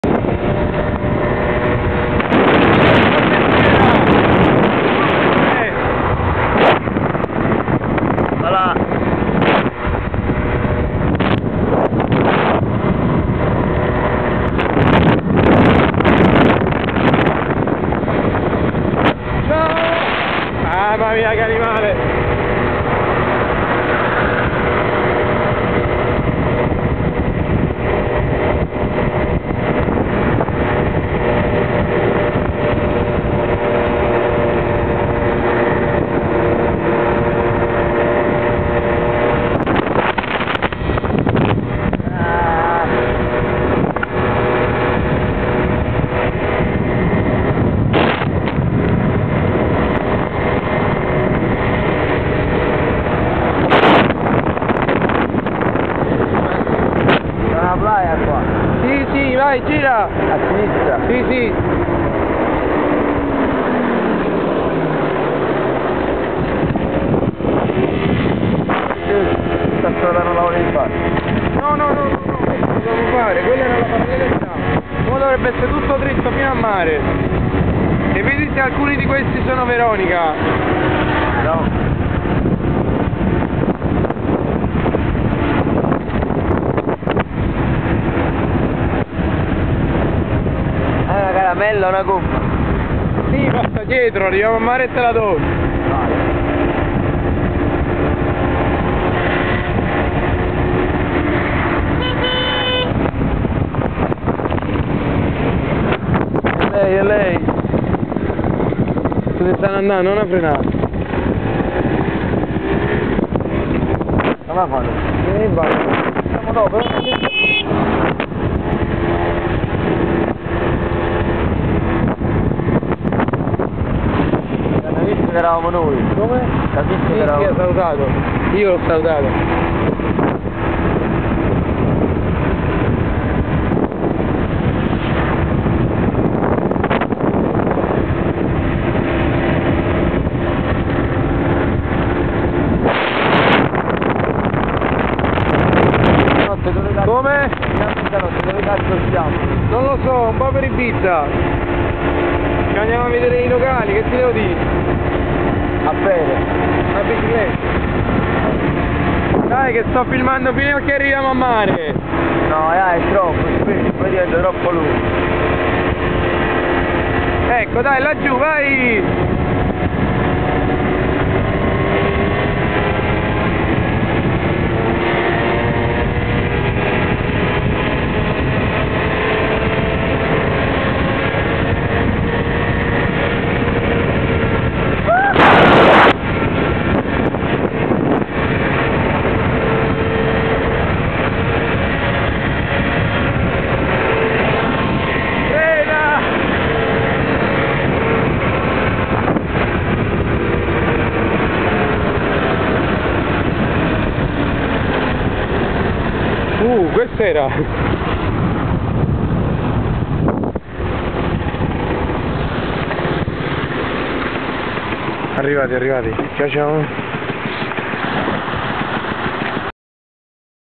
Formentera 09 - Corse folli sul cinquantino
Un pò di Formentera vista dal motorino! Direzione: Cà marì! ;-)